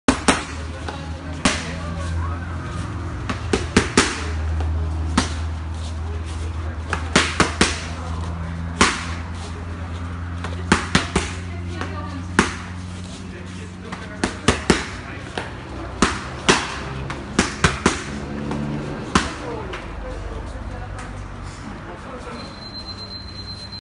Muay Thai & MMA - Box drill | Marilett Rodriguez & Manuel Montoya | Zap Jal Mexico 2016 | Tiger Thai
"Clases de Box, MMA, Muay Thai, Kick Boxing, Full Contact"
Maestros y profesores: Manuel Montoya C. & Marilett Rdz Pinedo. Tiger Thai MMA - México (31 Agosto 2016)